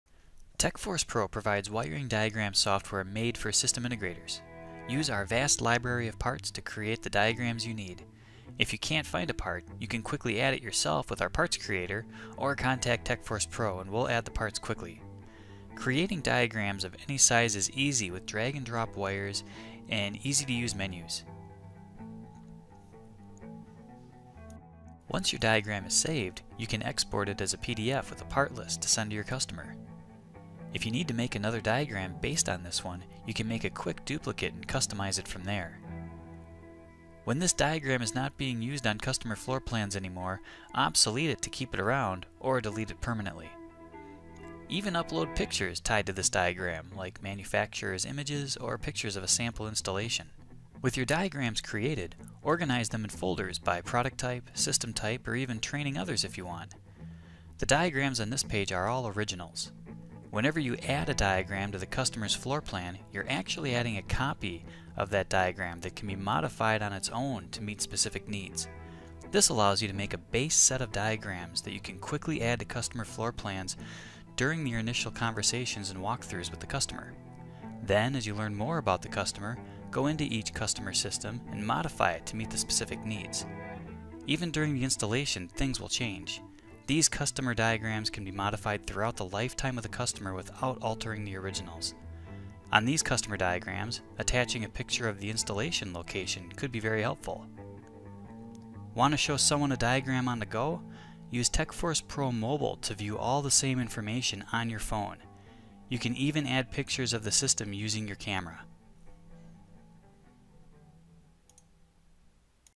Manage your System Diagrams with Techforce Pro
Watch this video to learn how Techforce Pro provides the best wiring diagram software available today.   Create wiring diagrams, add them to your customer floor plans, and use Techforce Pro Mobile to share them with your team.